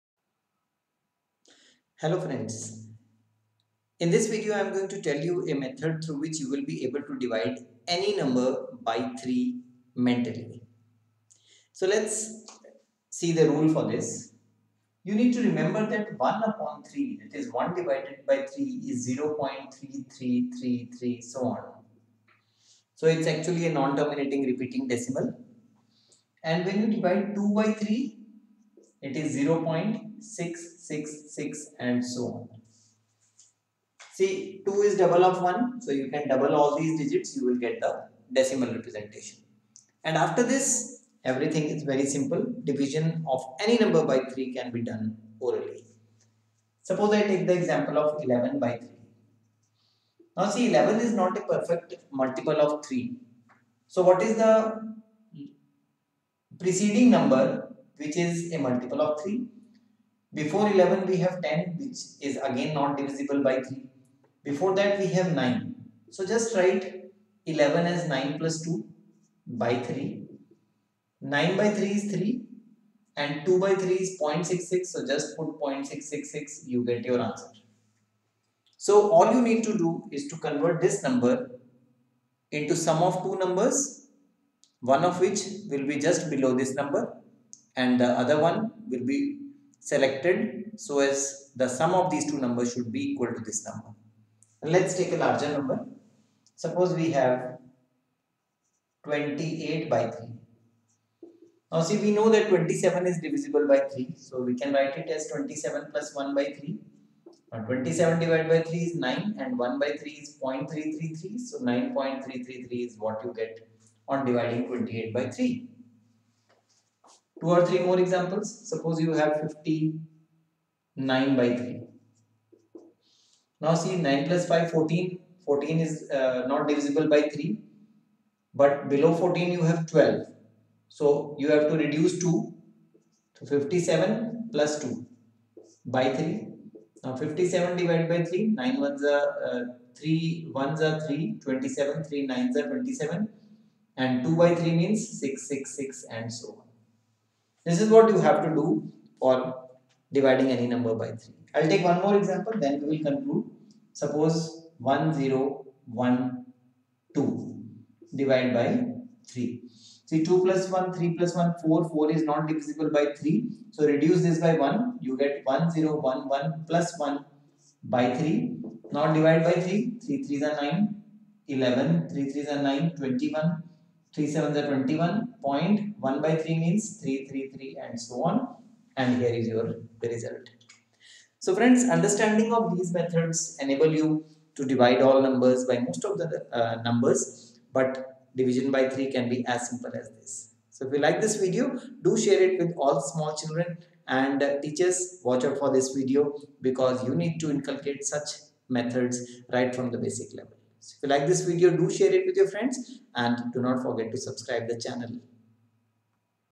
Divide Any Number by 3 Mentally in 3 Seconds
Shortcuts to do calculations mentally, useful for developing numerical aptitude and for success in competitive examinations. division can also be done using some easy short tricks. watch this video to learn how to divide any number by 3 mentally in just 3 seconds. for more videos, keep visiting: